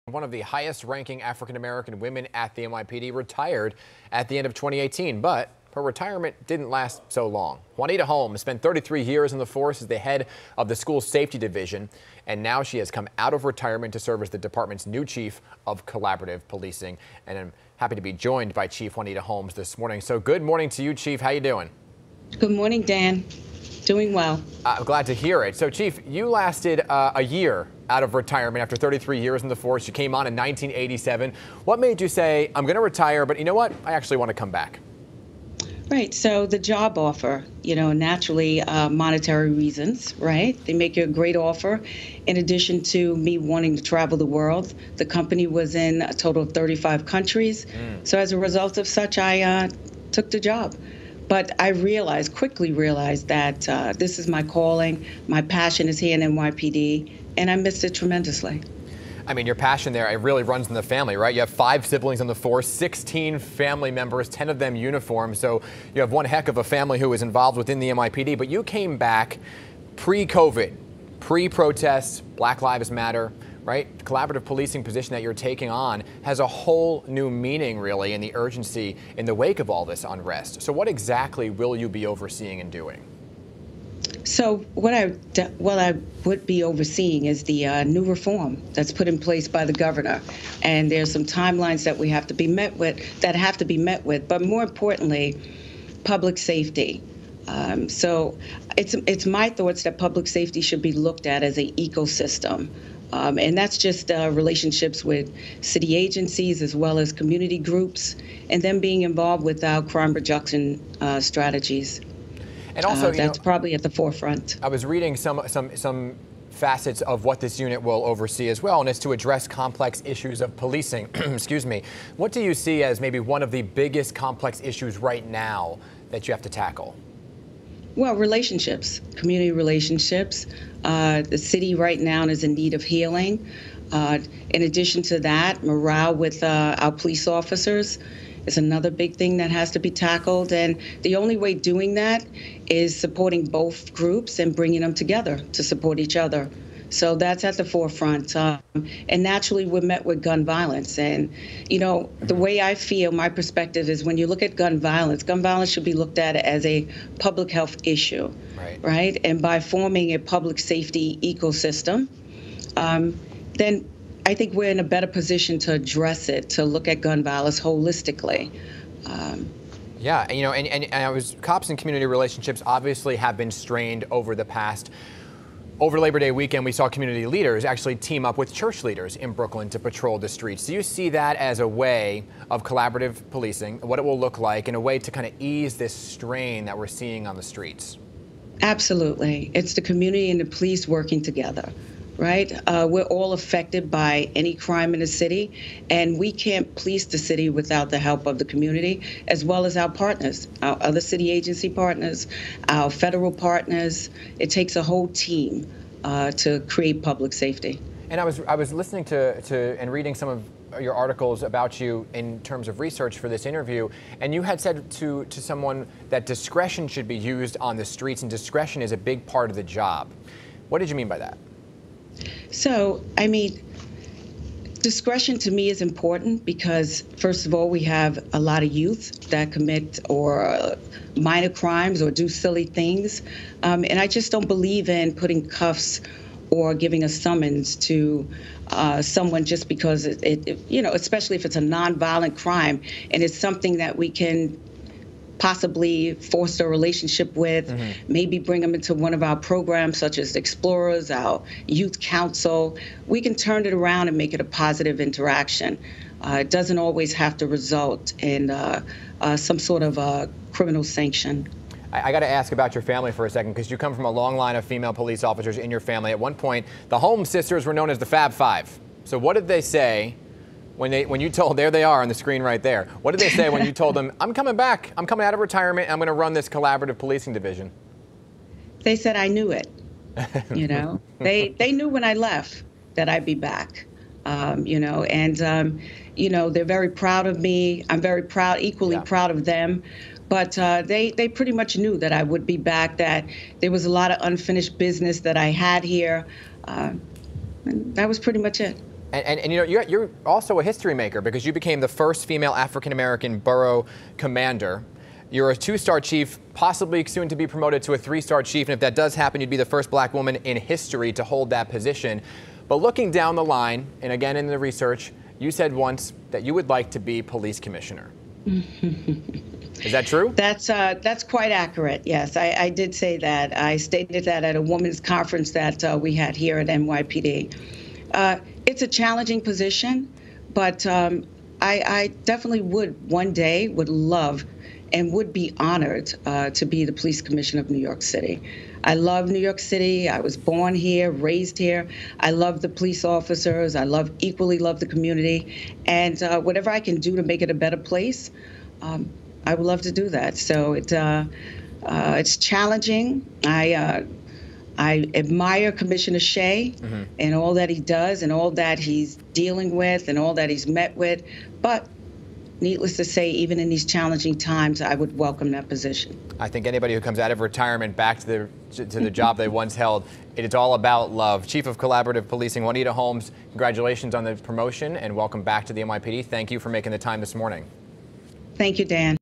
NYPD chief explains collaborative policing
Assistant Chief Juanita Holmes has been named the new chief of collaborative policing.